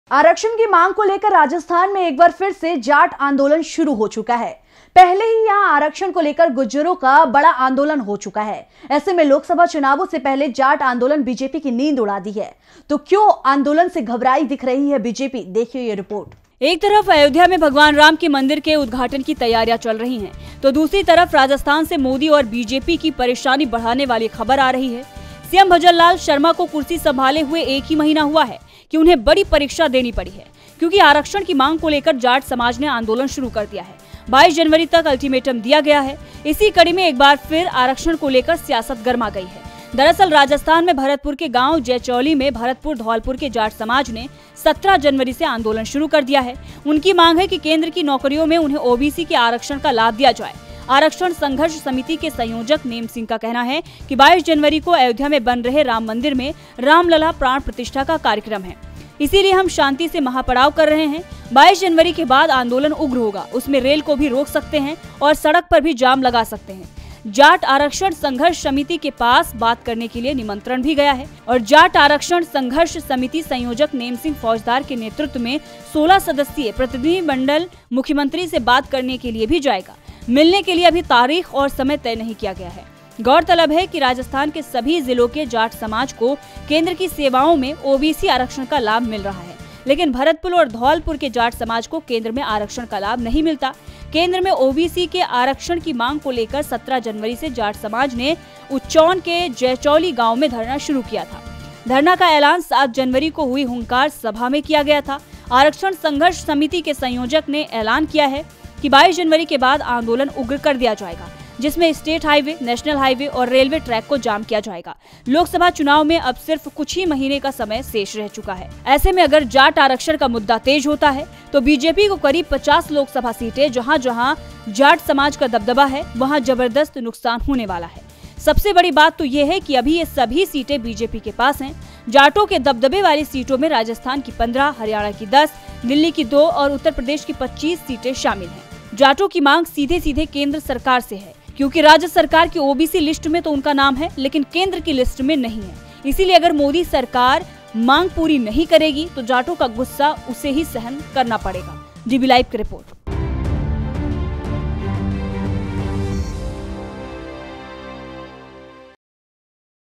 22 जनवरी के बाद जाट आरक्षण पर होगा बड़ा खेल | रेल और सड़क मार्ग को रोकने की धमकी | Ram Mandir
22 जनवरी के बाद जाट आरक्षण पर होगा बड़ा खेल | रेल और सड़क मार्ग को रोकने की धमकी | Ram Mandir